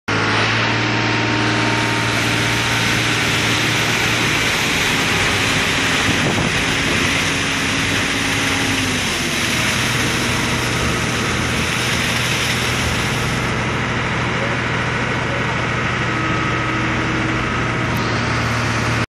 Chardonnay Grapes Offloaded at Zandvliet Estate
2015 Chardonnay grapes being offloaded into the crusher and destemmer.